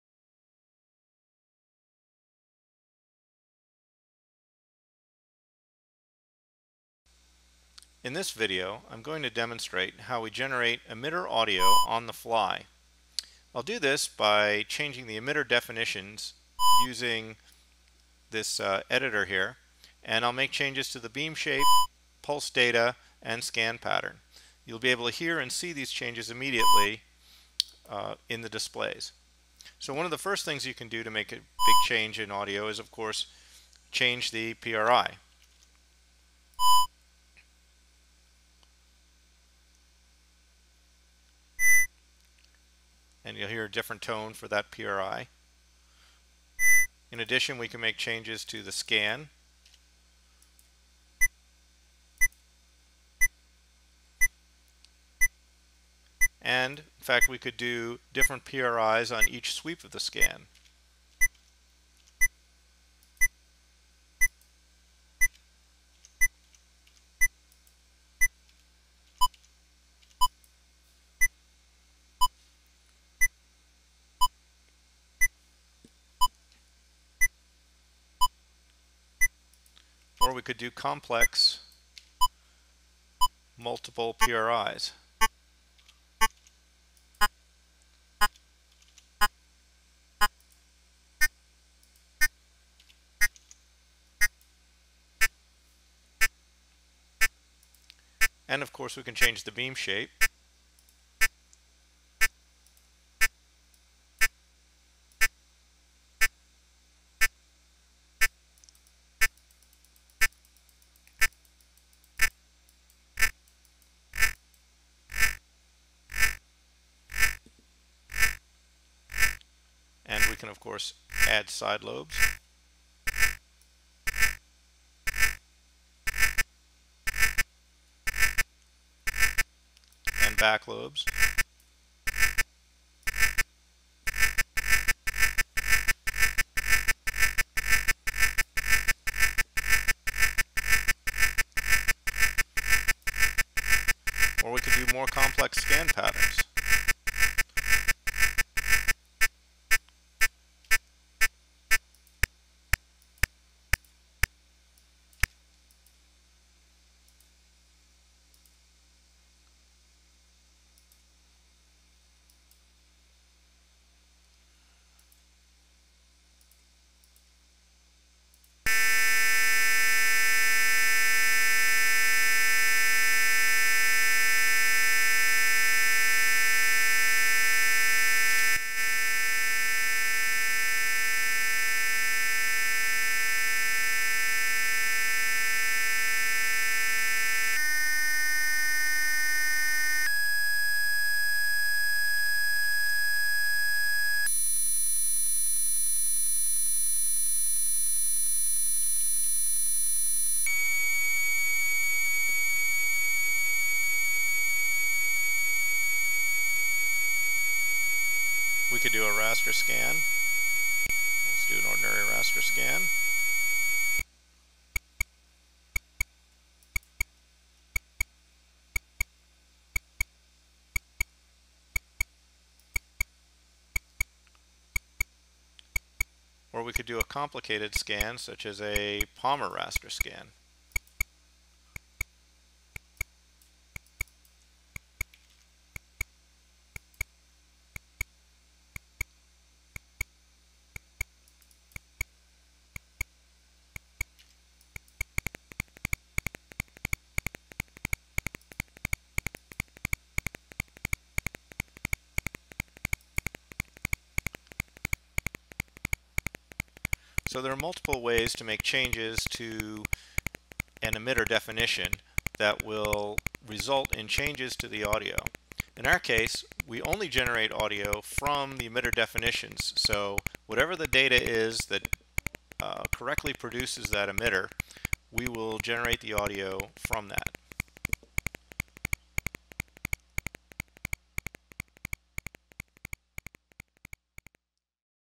Emitter Audio Generation
An overview of BSI's new Signal Generation Engine (SGE) including the user interface for defining beam, scan and pulse patterns and the corresponding emitter audio generation.